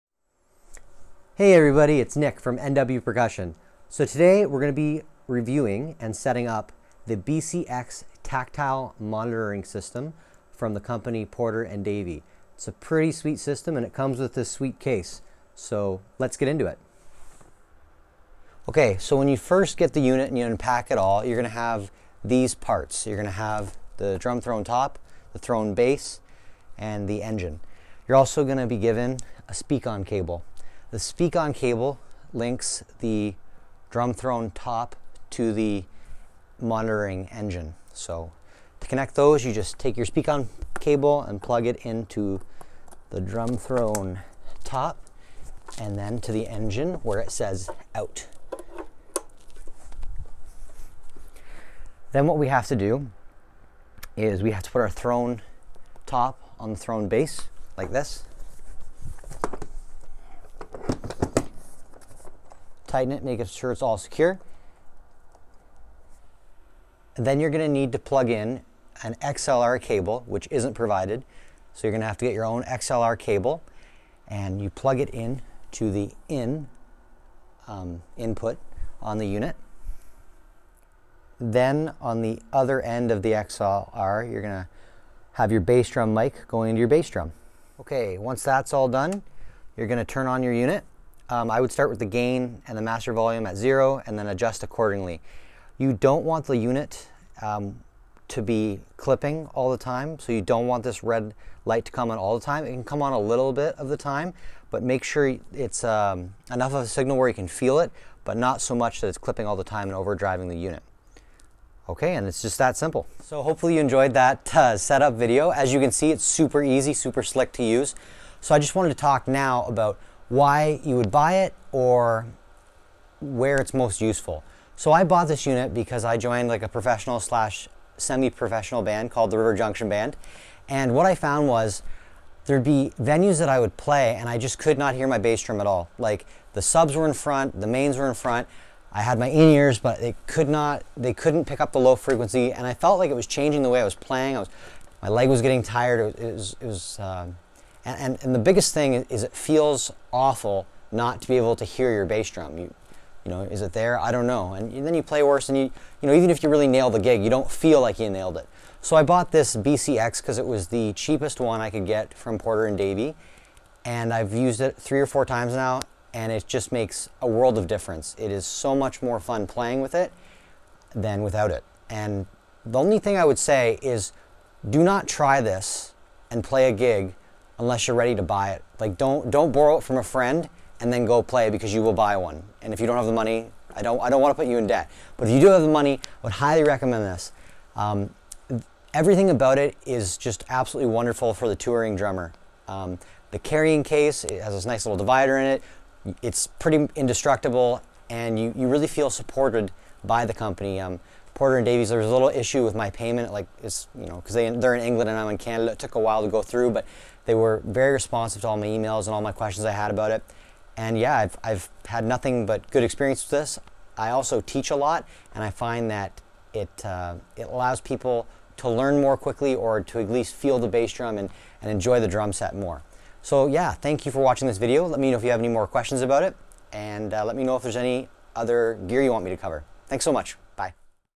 BC X PORTER AND DAVIES - Step by step set up and review
This is a pretty sweet unit that allows you to feel your bass drum as if it were  a part of your body! Any questions? Please ask :)